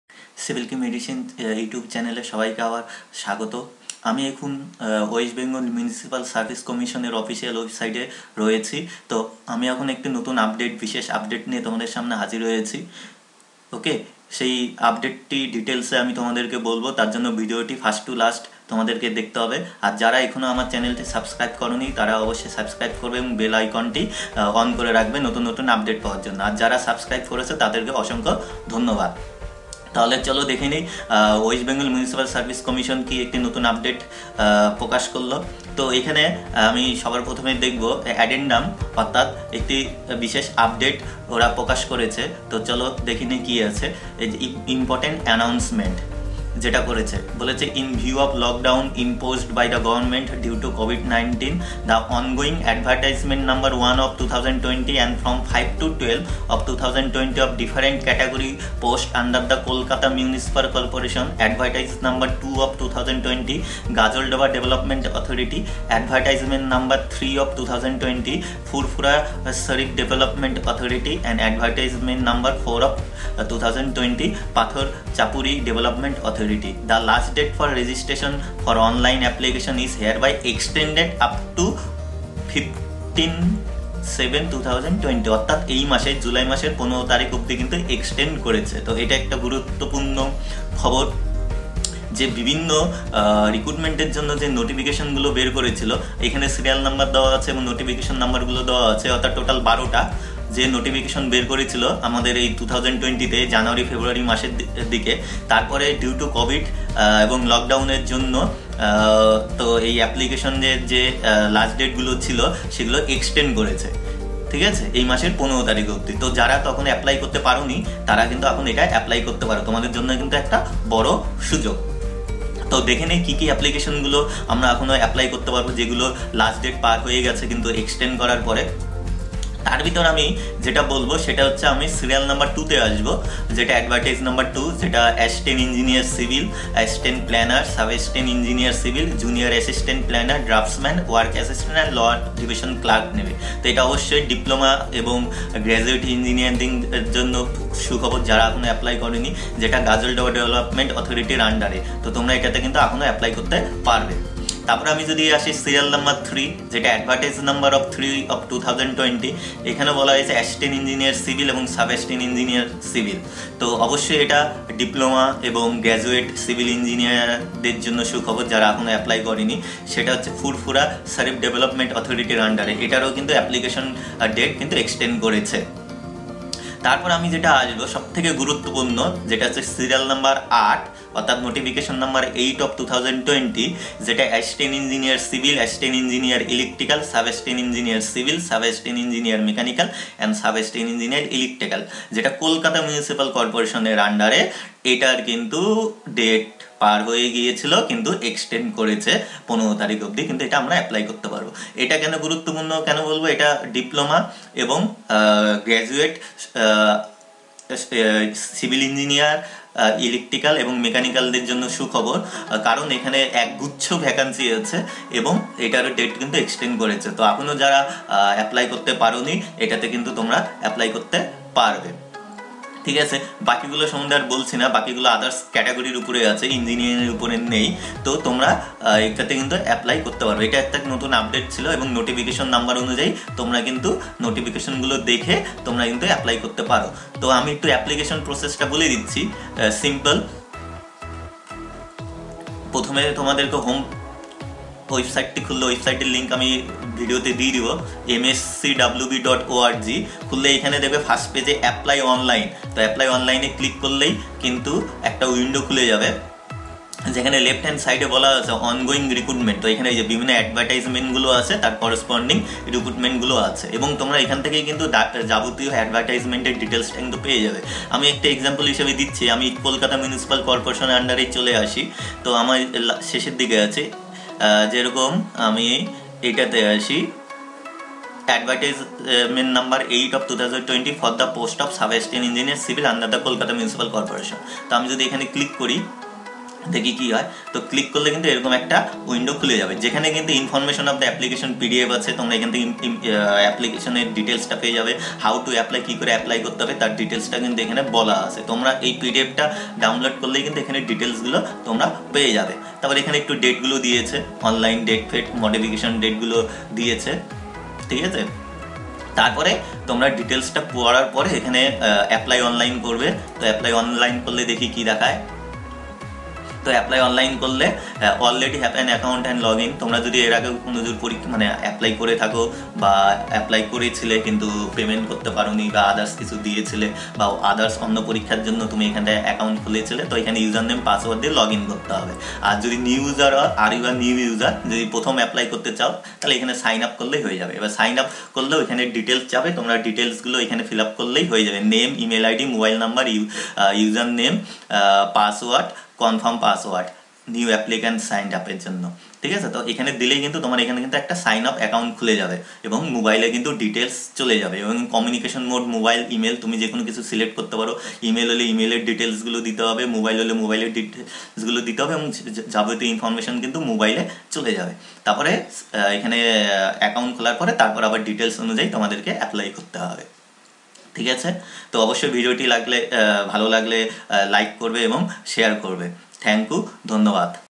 MSCWB NEW UPDATE II DATE OF VARIOUS APPLICATIONS EXTENDED
WEST BENGAL MUNICIPAL SERVICE COMMISSION RELEASED A NEW UPDATE REGARDING THE ONGOING APPLICATION OF VARIOUS POSTS.
DATE OF APPLICATION IS EXTENDED UP TO 15TH JULY,2020.
OFFICIAL WEBSITE OF WEST BENGAL MUNICIPAL SERVICE COMMISSION -
2. INSTAGRAM-
3. FACEBOOK-

GOOD BYE AND TAKE CARE.STAY HOME,STAY SAFE.